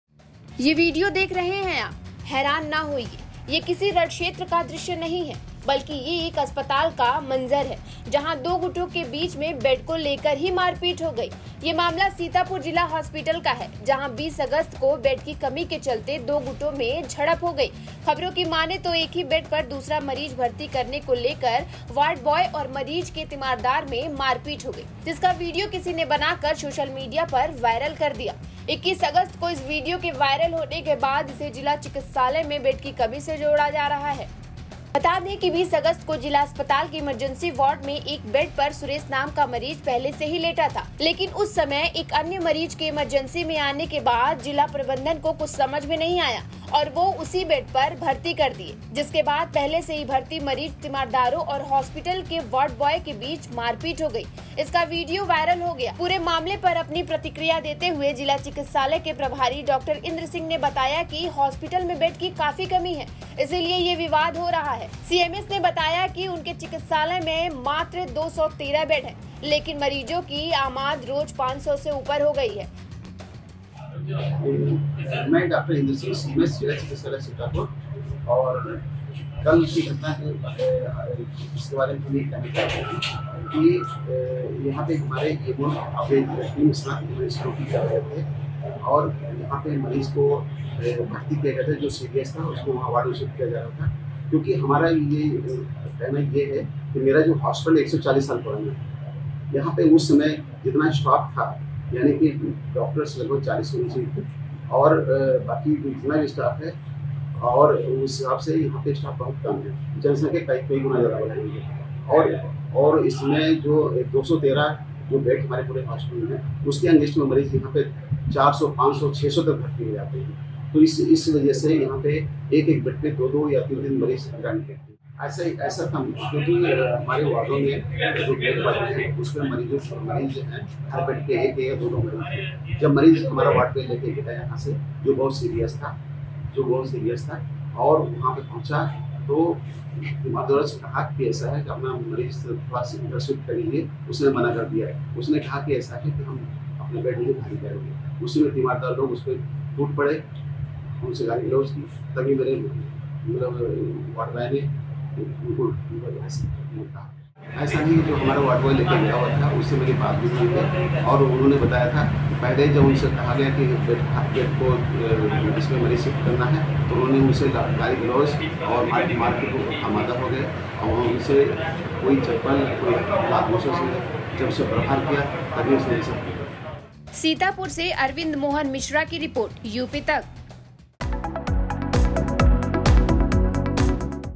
Sitapur Viral News: इलाज कराने गए थे, अलग ही इलाज हो गया!
Sitapur Viral News:  ये मामला सीतापुर जिला हॉस्पिटल का है, जहां 20 अगस्त को बेड की कमी के चलते दो गुटों में झड़प हो गई. खबरों की माने तो एक ही बेड पर दूसरा मरीज भर्ती करने को लेकर वार्ड बॉय और मरीज के तीमारदार में मारपीट हो गई.  जिसका वीडियो किसी ने बनाकर सोशल मीडिया पर वायरल कर दिया.  21 अगस्त को इस वीडियो के वायरल होने के बाद इसे जिला चिकित्सालय में बेड की कमी से जोड़कर देखा जा रहा है...